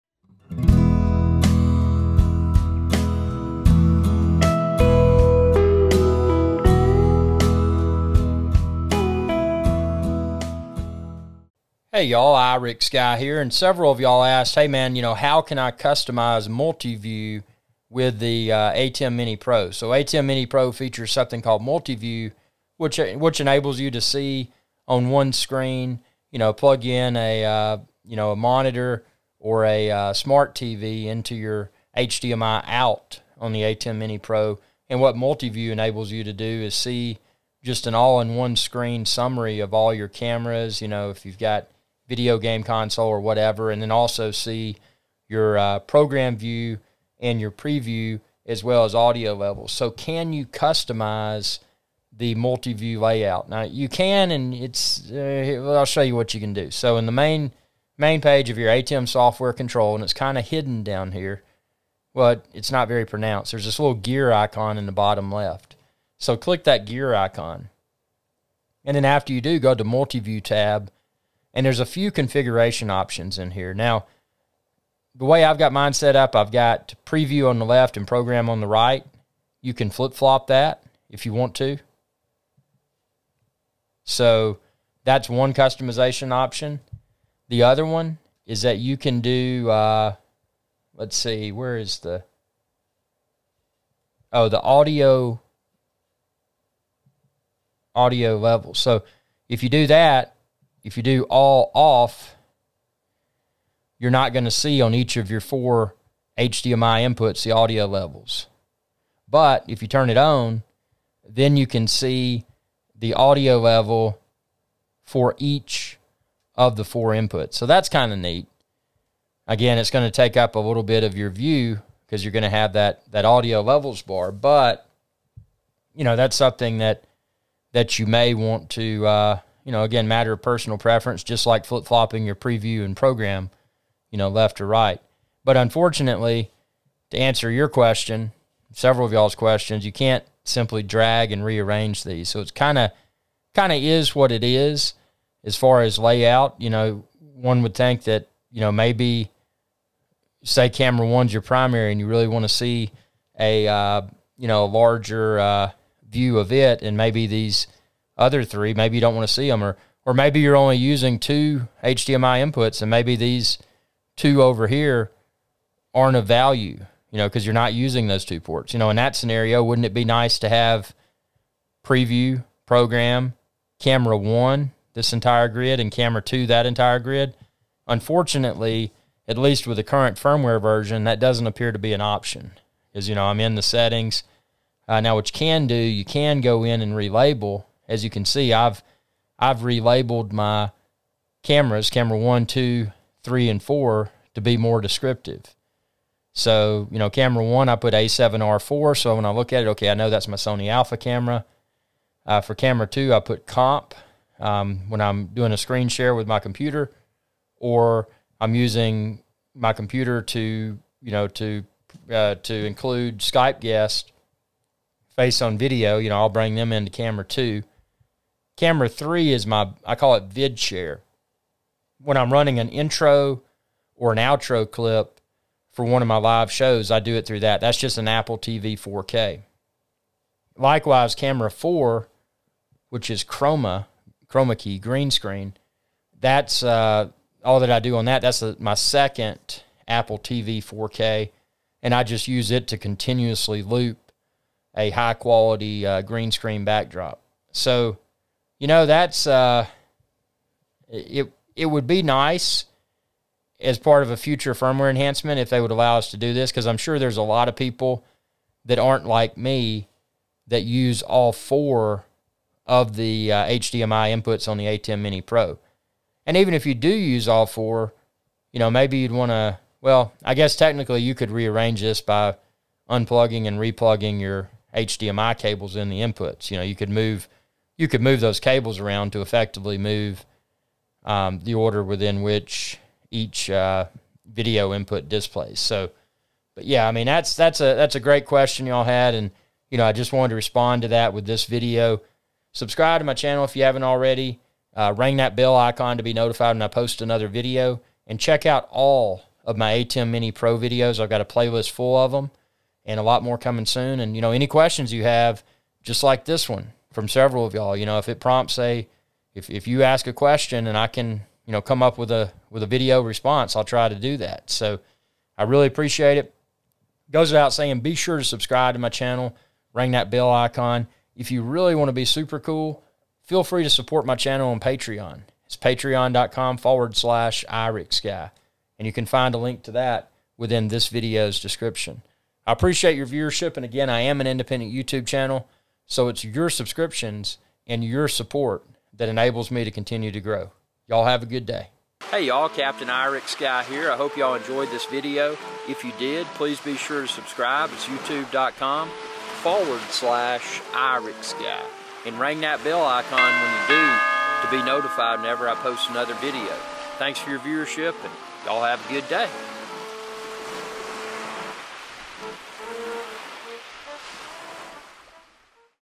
ATEM Mini Pro - Customize Multiview
With the ATEM Mini Pro firmware current as of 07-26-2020, these are the Multiview customization options that are available. Within this video, I demonstrate which ATEM Mini Pro Multiview layout settings can be changed, as well as vocalize requests for future Multiview enhancements. Enjoy and share with others too!